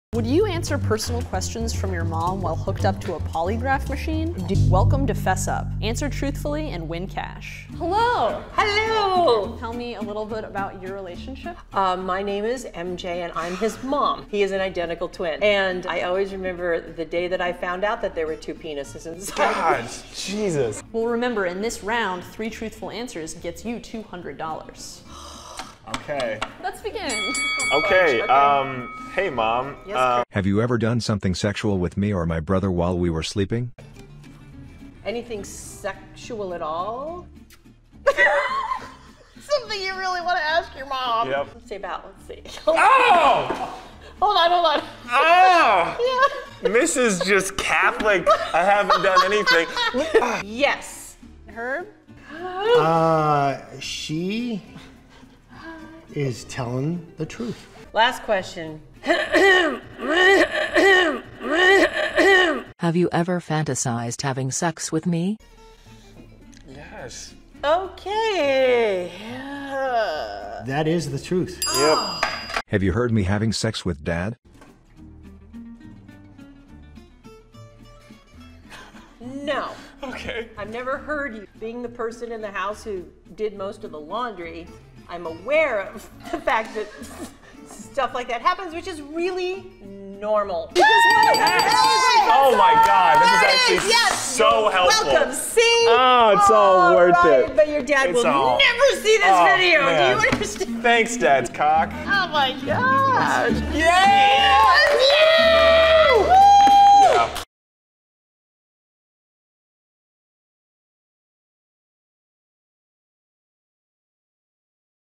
Mom Reveals Lifelong Secrets to Son On Lie Detector Test (Gone Sexual) (Gone Wrong)
Lie detector test mom and son edited to be more funny lol
Polygraph test super funny gone sexual and gone wrong you will laugh no matter what